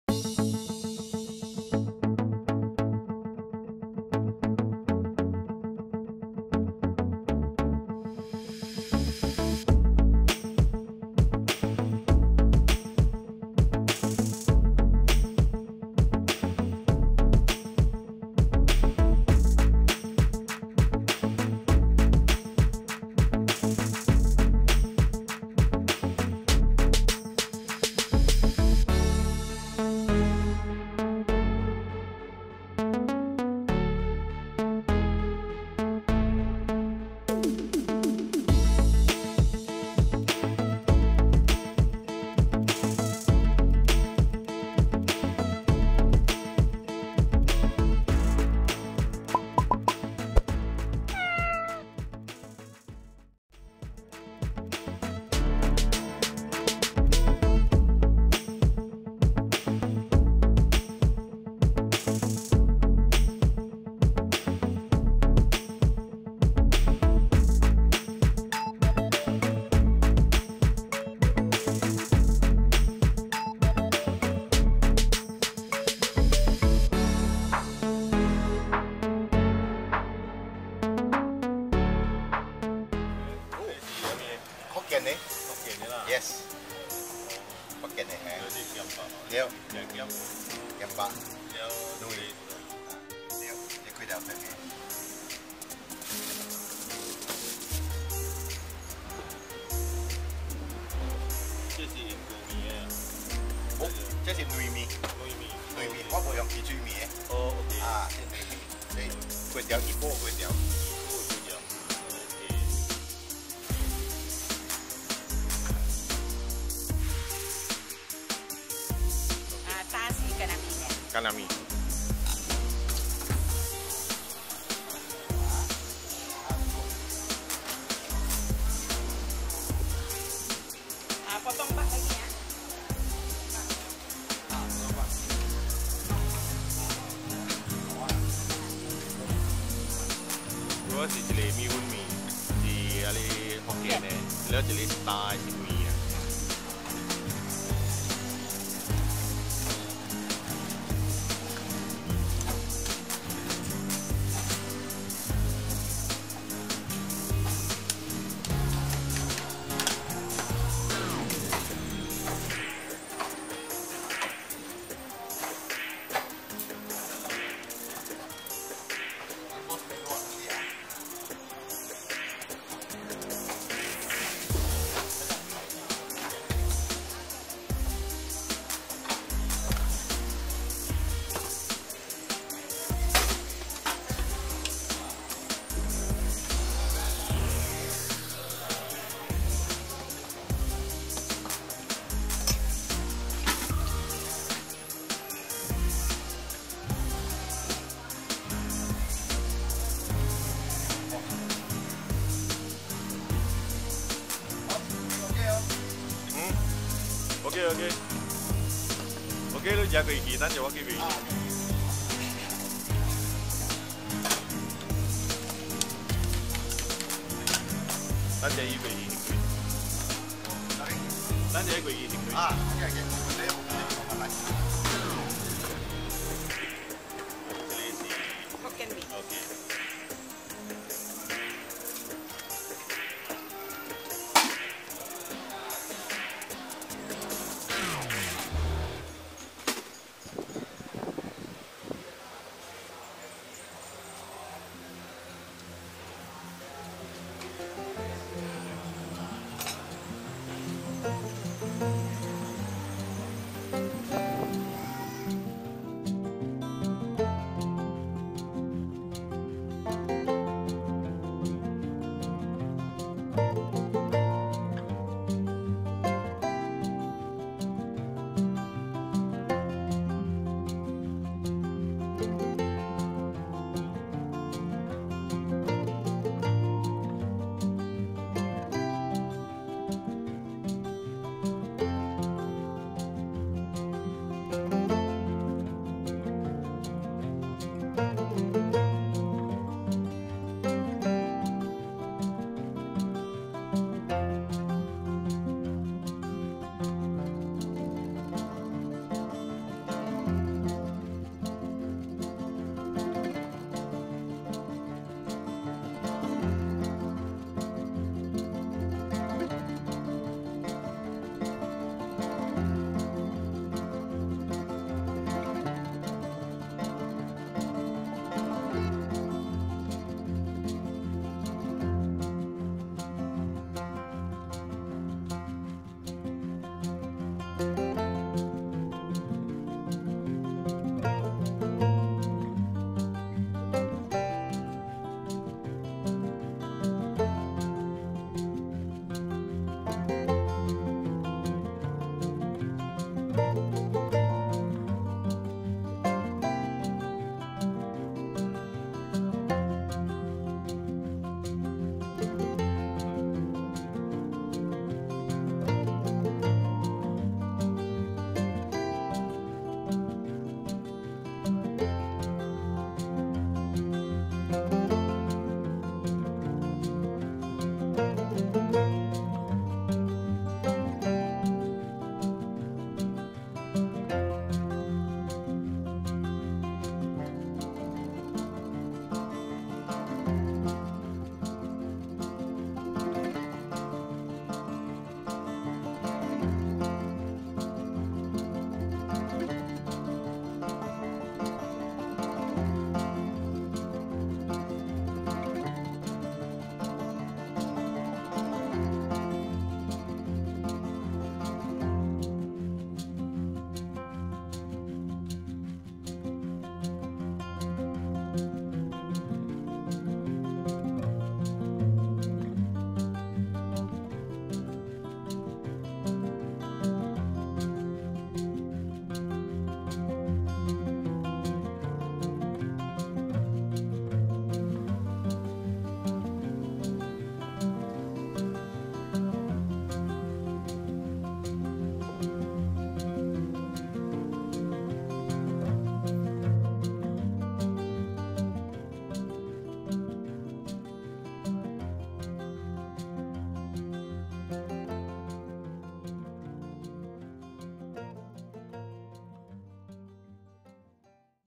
EP96 佳乡岭饮食中心咸肉干面一级棒,福建虾面也不错Salt Pork Dry Noodles & Hokkien Noodles Delicious Malaysia SP Street food
佳乡岭饮食中心咸肉干面一级棒,福建虾面也不错
Salt Pork Dry Noodles & Hokkien Noodles Delicious

Like Facebook Page 点赞面子书专页:SP Exploring Street Food 小猫探店 @xiaomaotandian

Lagenda Height Food Court (佳乡岭)- 福建面Hokkien Noodles
186, Jalan Legenda 7, Lagenda Heights, 08000 Sungai Petani, Kedah
business hours : 6.45am-11am (Thursday closed)